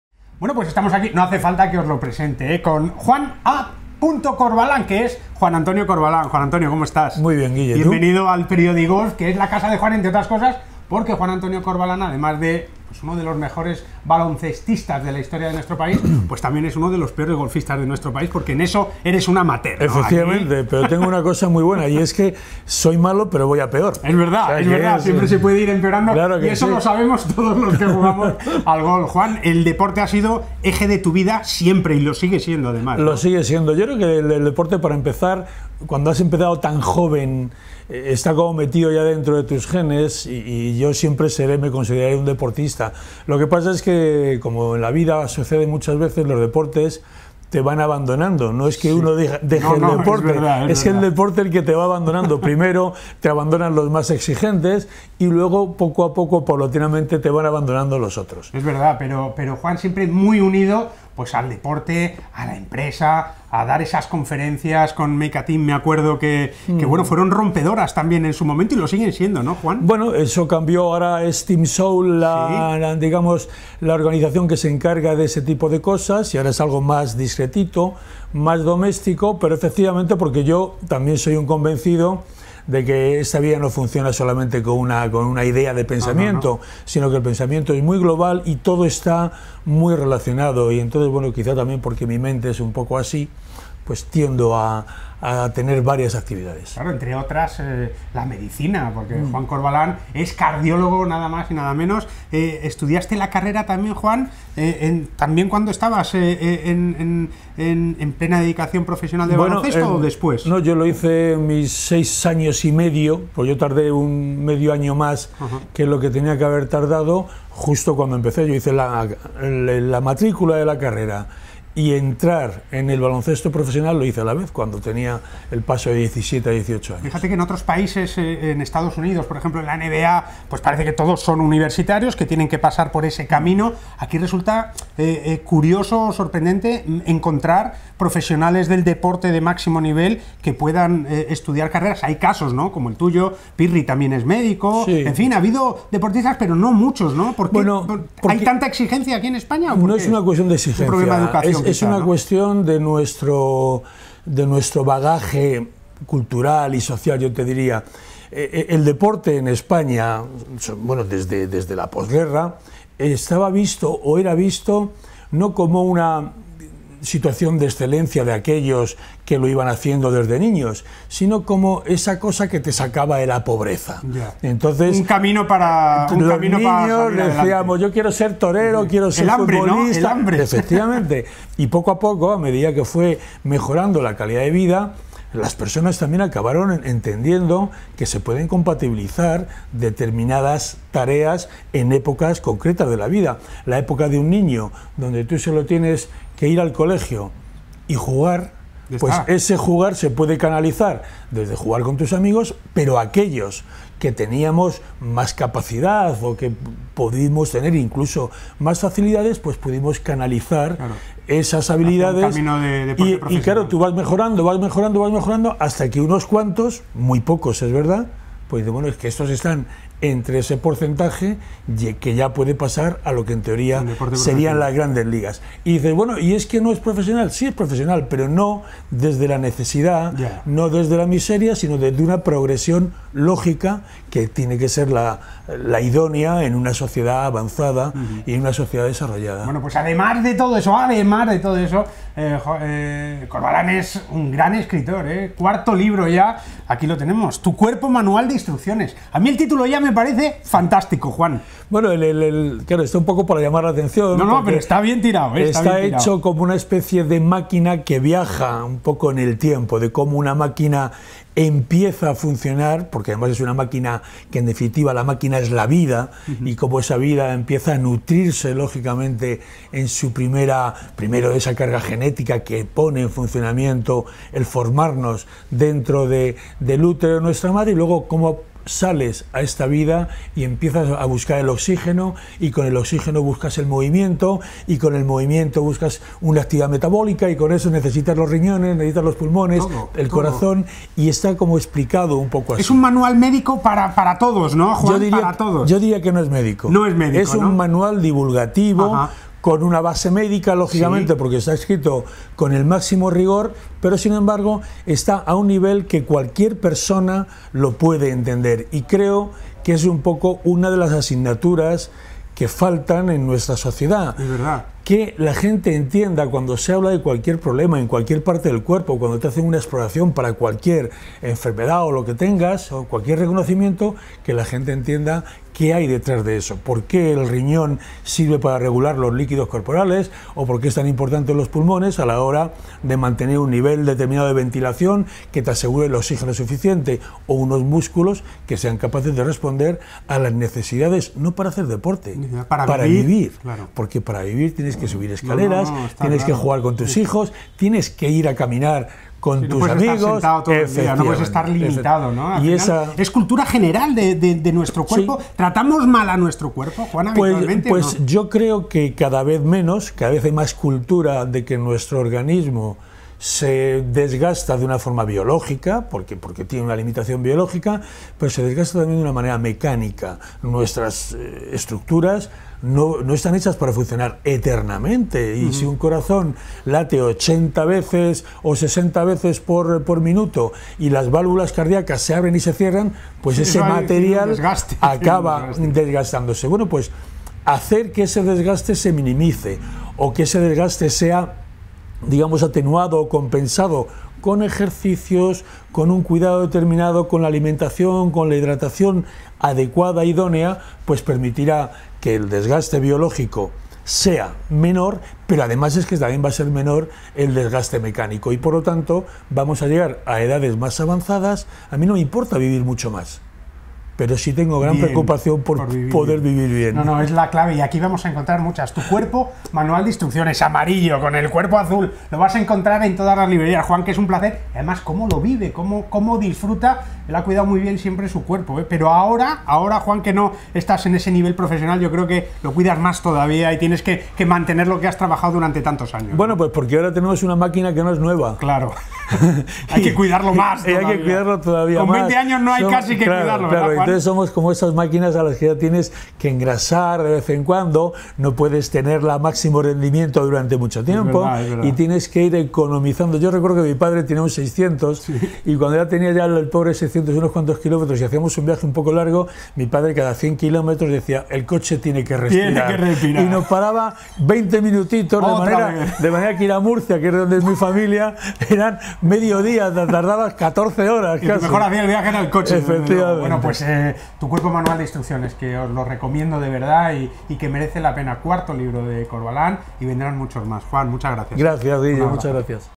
Juan Antonio Corbalán, autor de 'Tu Cuerpo, Manual de instrucciones'
Guillermo Salmerón entrevistó a J. A. Corbalán que nos vinó a hablar de su nuevo libro: 'Tu cuerpo, Manual de instrucciones'.